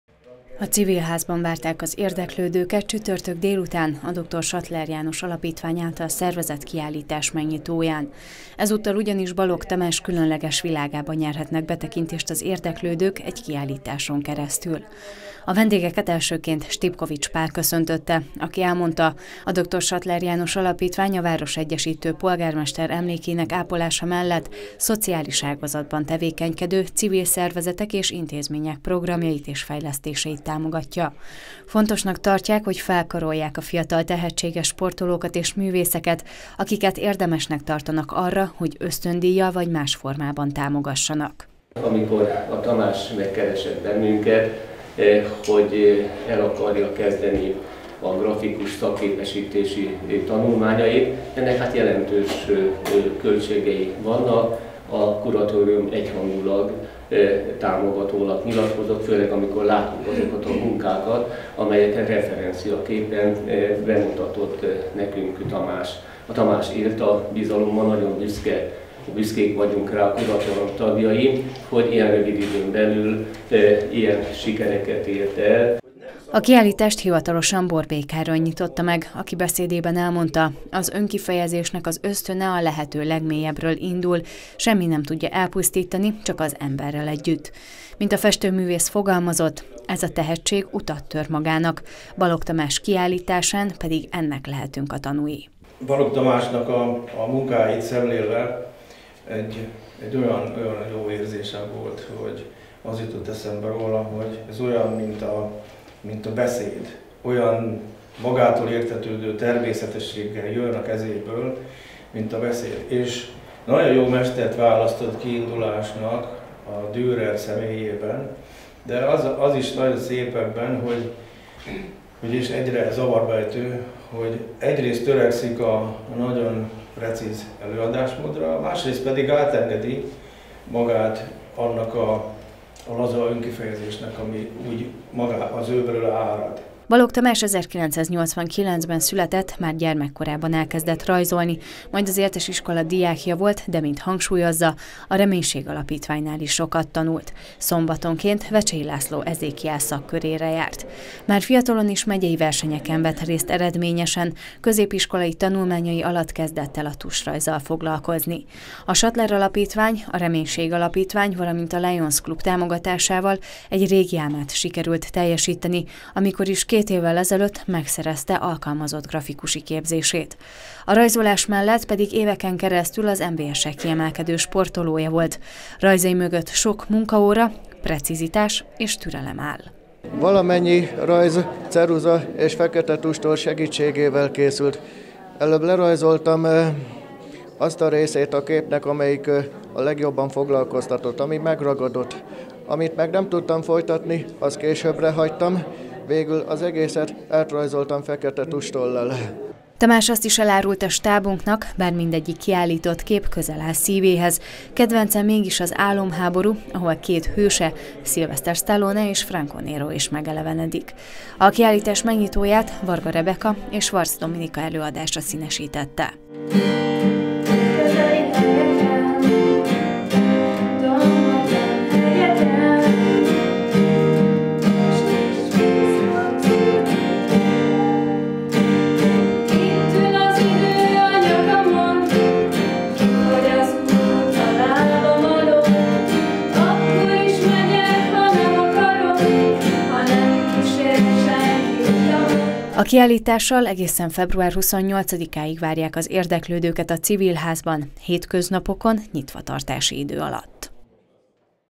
Balogh Tamás munkáiból nyílt kiállítás a Civil-házban
A nemrégiben felújított és átadott Civil-ház biztosít otthont egy új kiállításnak, amit hivatalosan csütörtökön nyitottak meg a közönség előtt. Ezúttal Balogh Tamás mutatkozik be különleges képeivel.